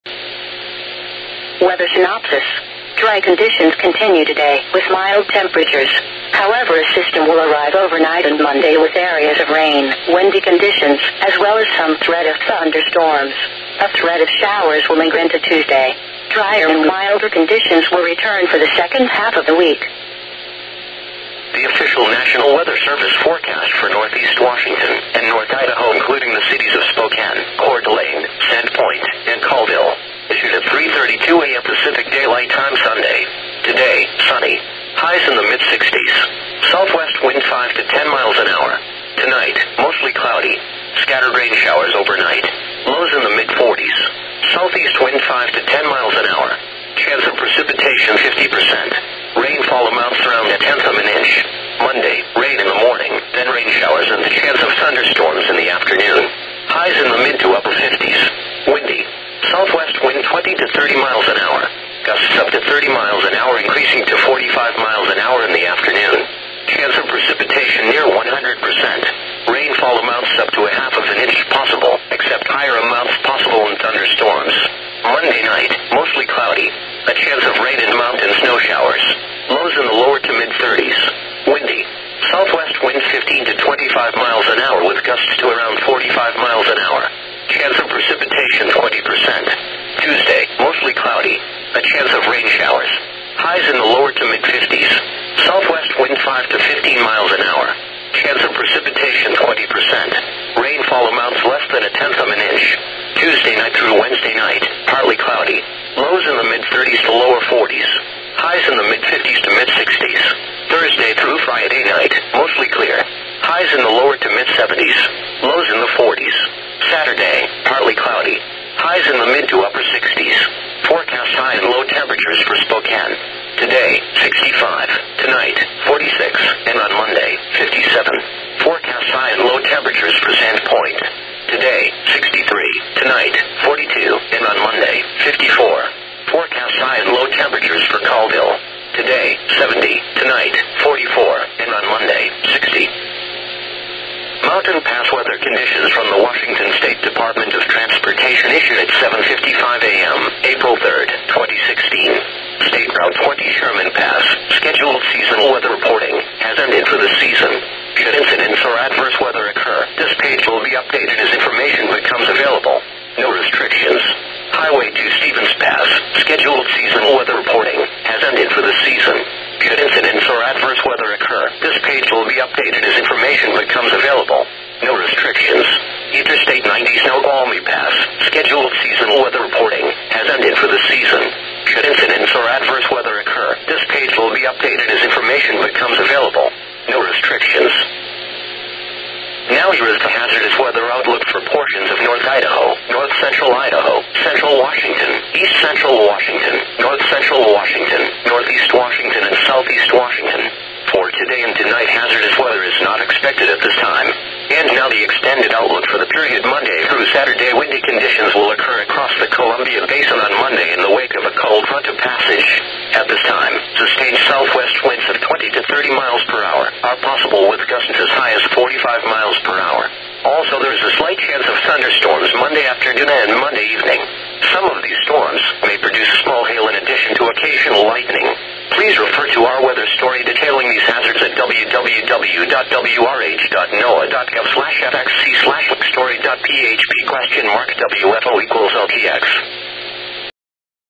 NOAA Weather Radio WXL86 Spokane WA CRS Broadcast Cycle April 3rd 2016
Taken from a stream archive on the wayback machine.